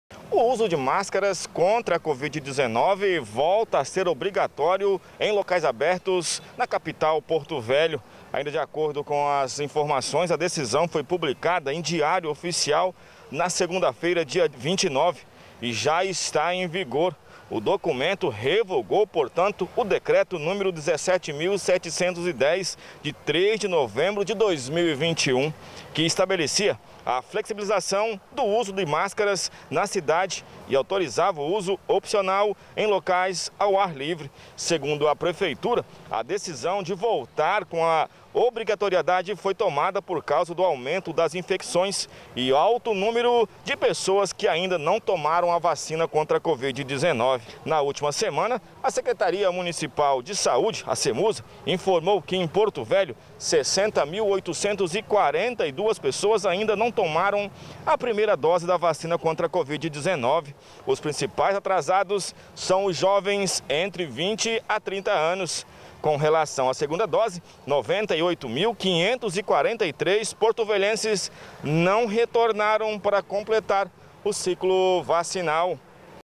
Uso de máscaras em locais abertos volta a ser obrigatório em Porto Velho
Créditos:
Reportagem: Mario Jensen
Cinegrafista: José Henrique
Edição: Mayke Júnior